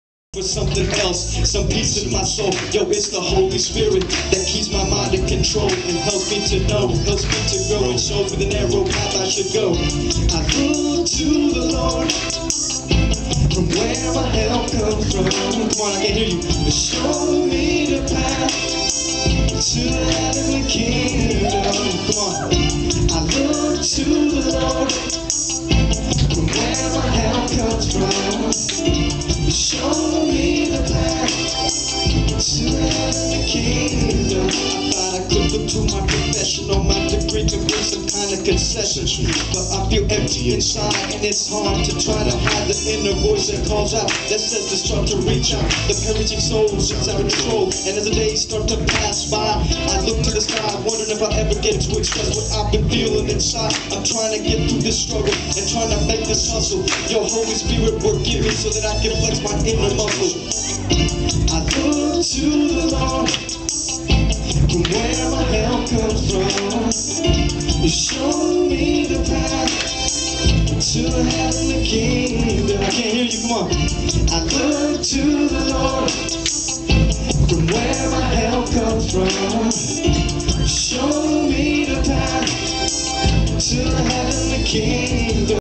Noble Solomon sings at 2007 SFCNA Conference
Noble Solomon sings at the 2007 SFCNA Conference in Oklahoma.